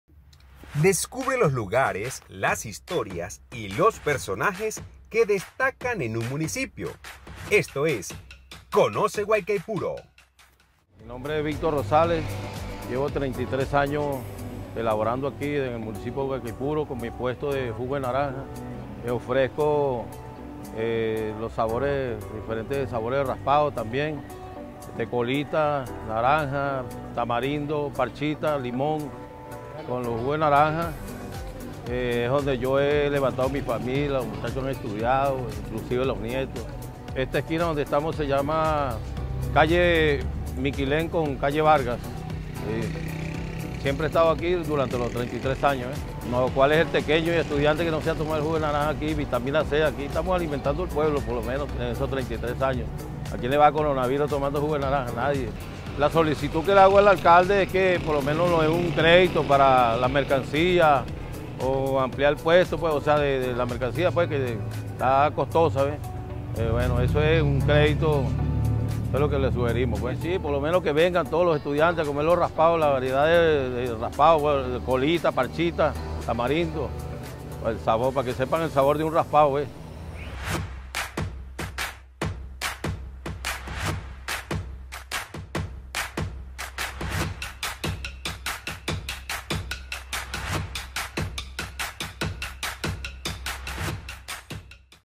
Emprendedores del Municipio Guaicaipuro-
Otro personaje relevante de nuestra Ciudad Capital es Victor Rosales, quien ha estado en el emprendimiento municipal por más de 33 años, síguenos en todas nuestras redes sociales y no te olvides de suscribirte a nuestro canal de YouTube para saber más de nuestro Municipio Capital.